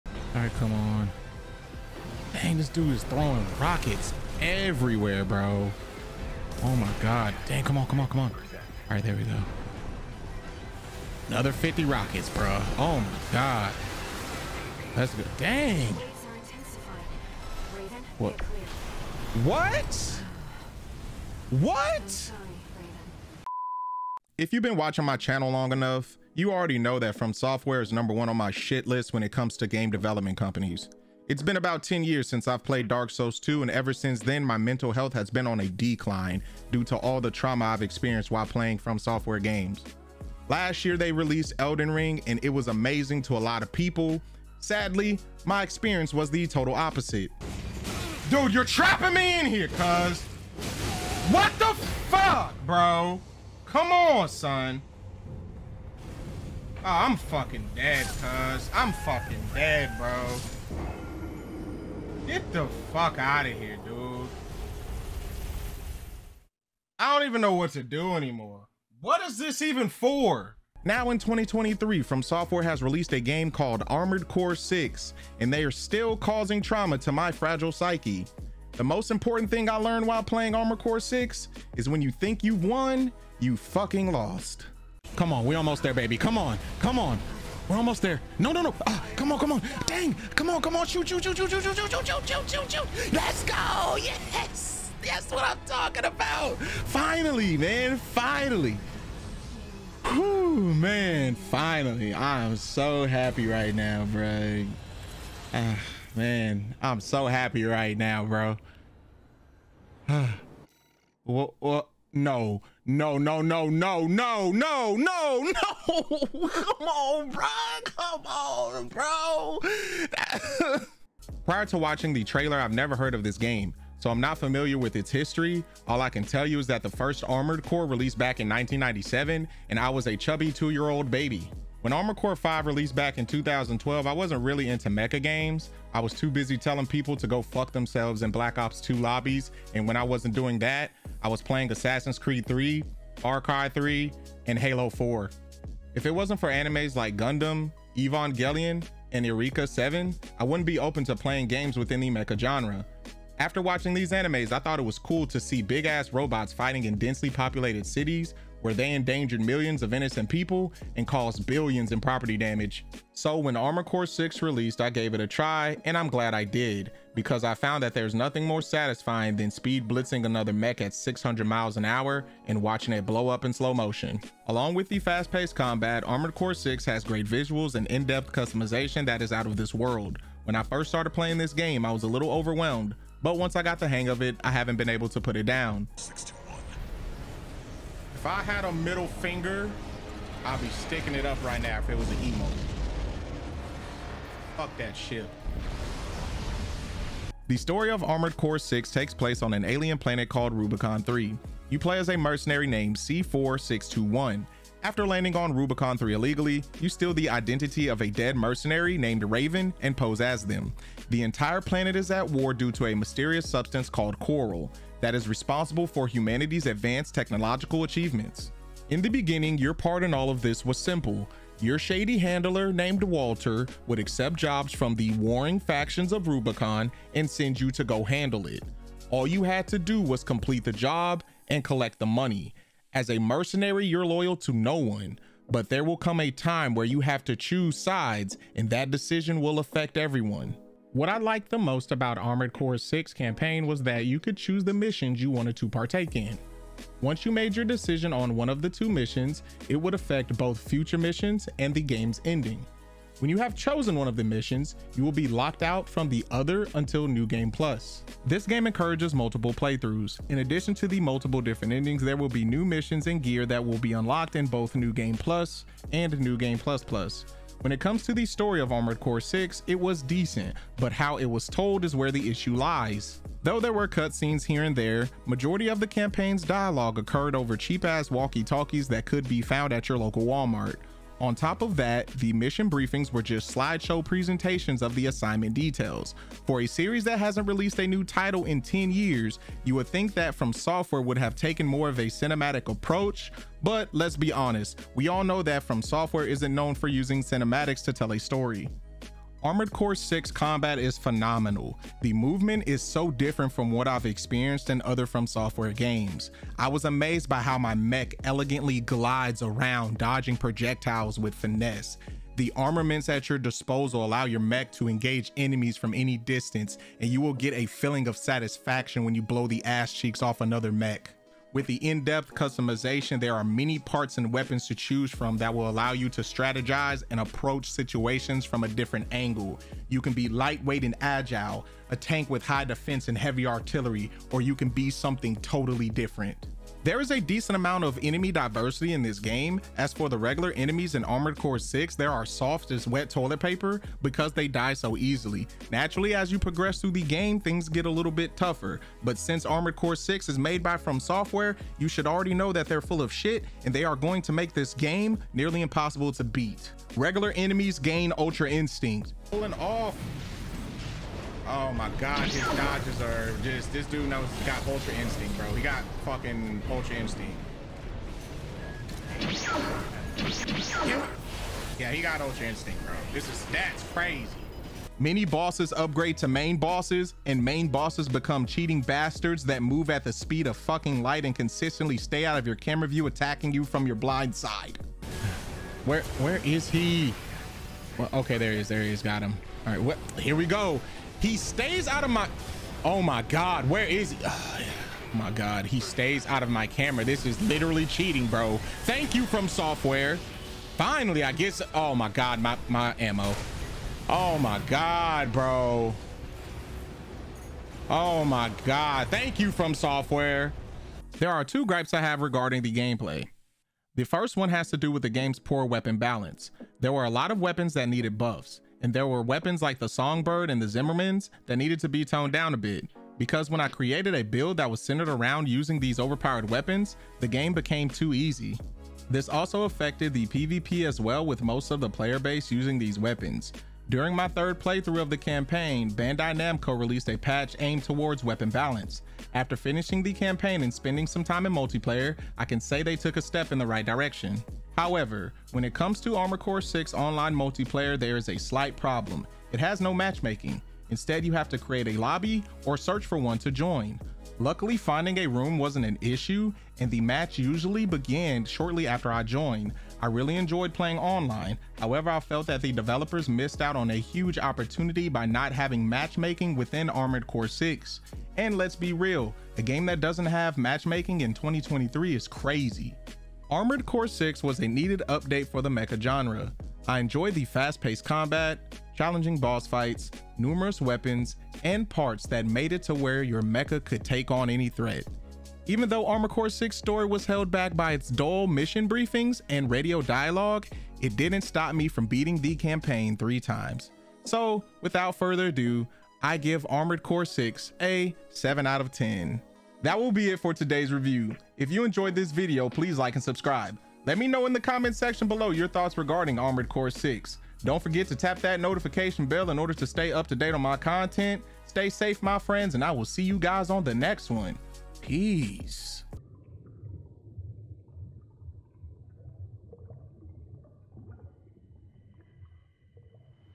Should Armored Core Have Given Us More? | Armored Core VI Review
Armored Core VI is a awesome mech game that everyone should try out. Make sure you have a psychiatrist because this game will give you bad thoughts.